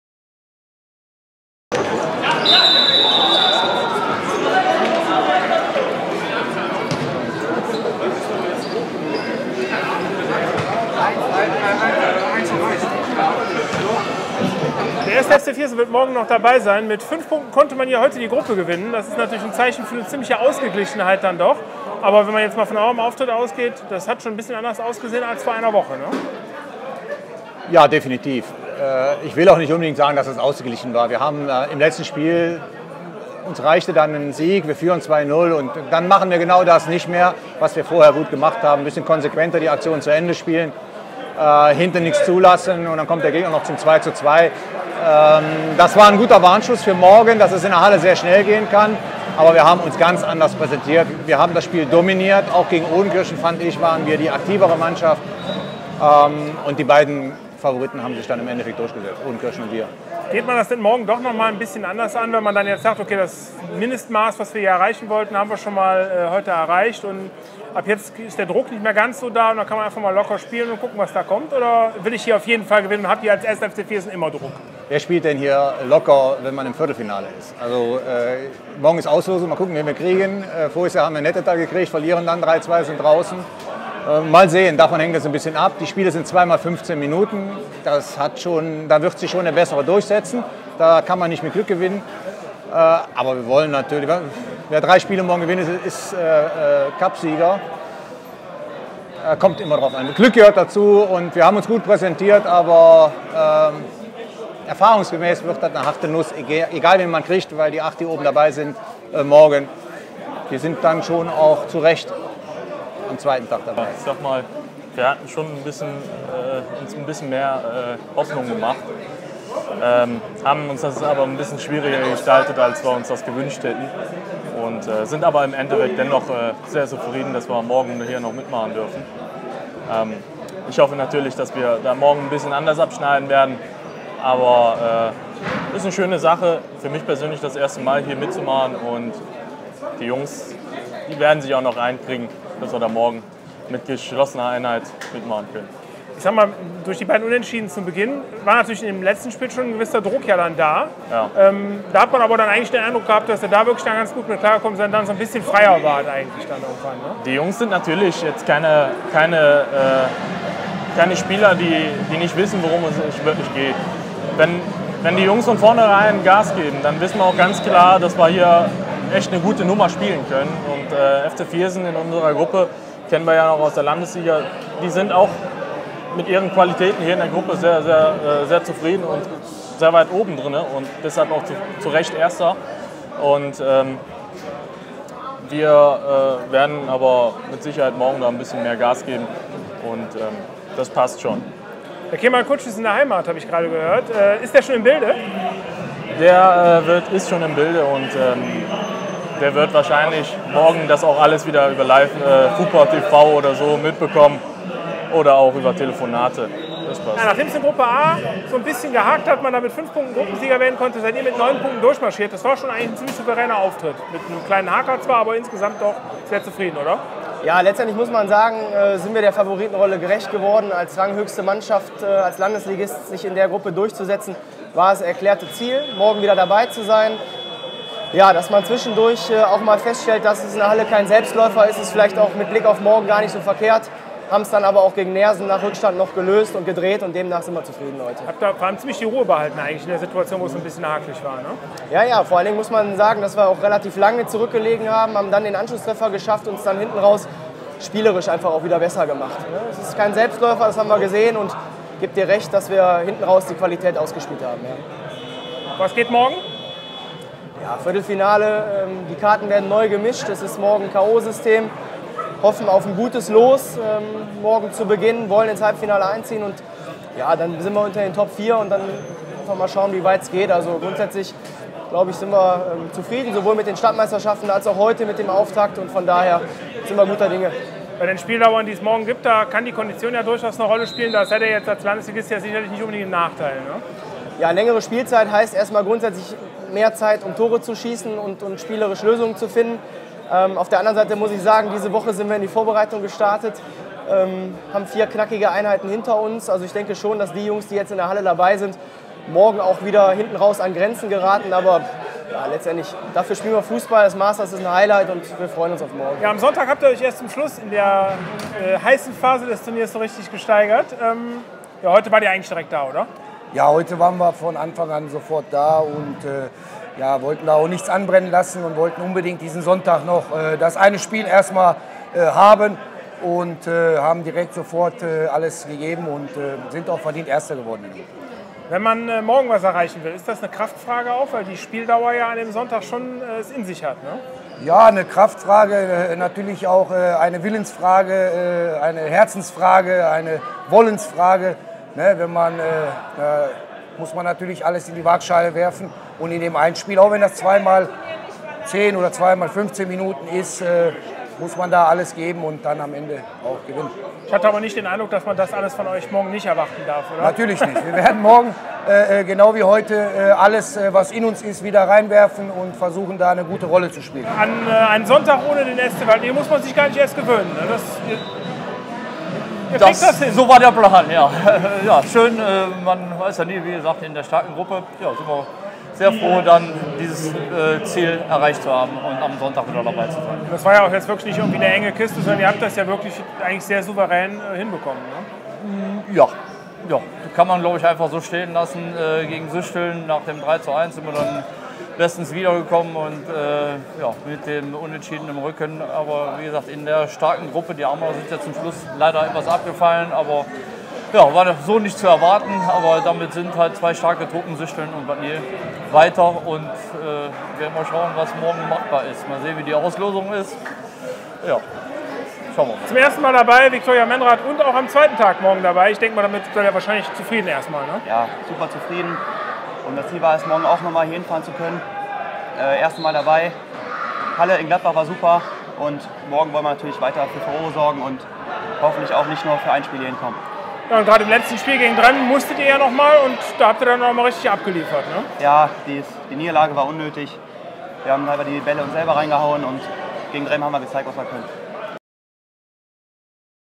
Stimmen Dülkener Hallenmasters 1. Tag (21.1.17)
Nach den vier Vorrundengruppen gingen wir am Ransberg in Dülken für Euch auf Stimmenfang!